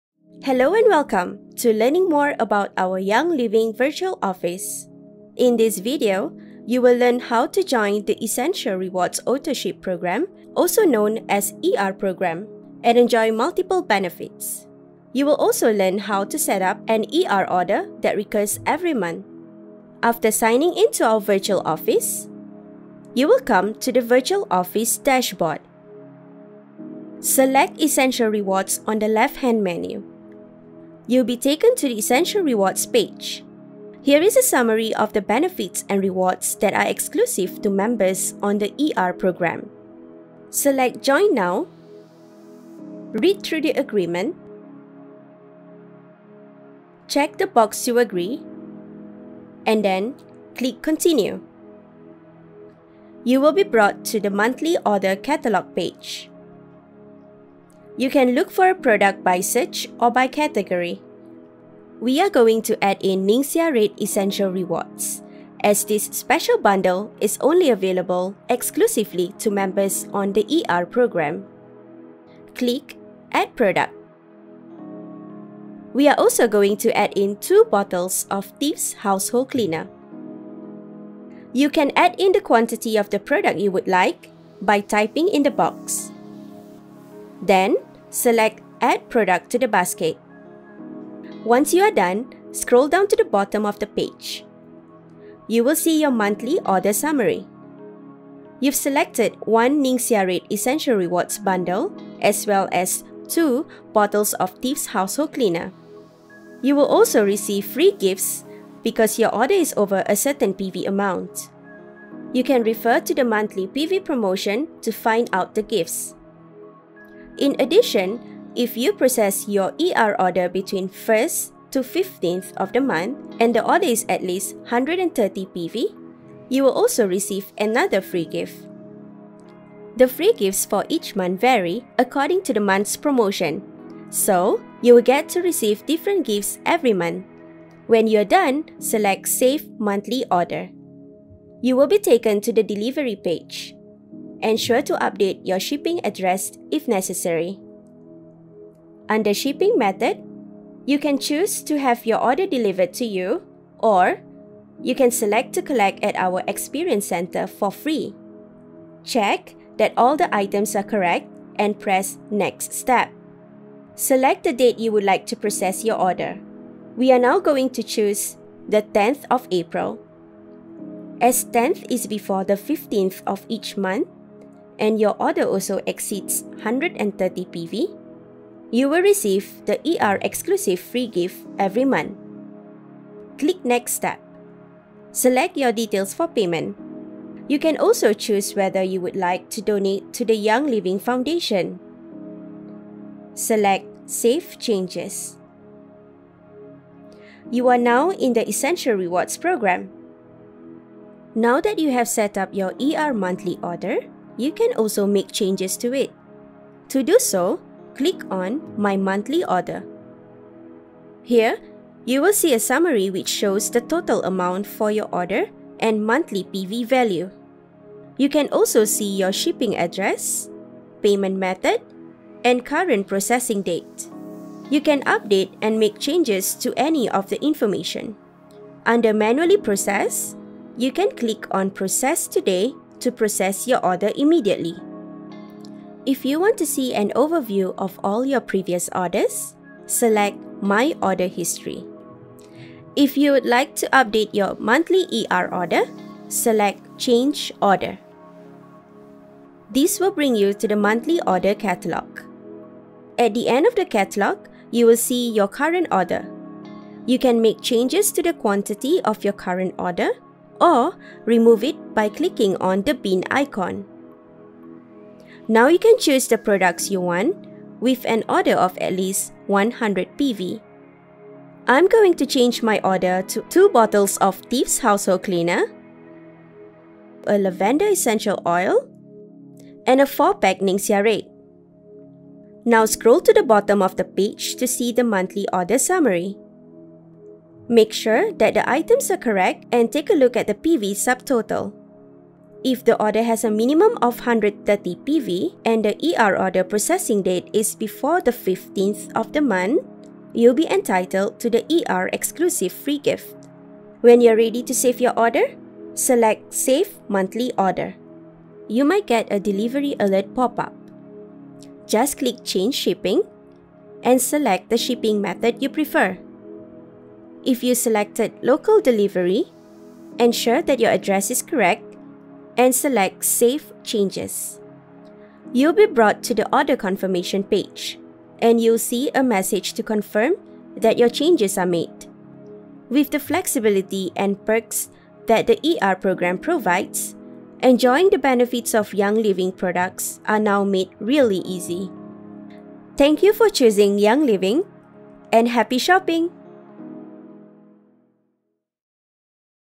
How to set up your Essential Rewards template on Virtual Office
Here's how to set up up and process your Essential Rewards order on Young Living Virtual Office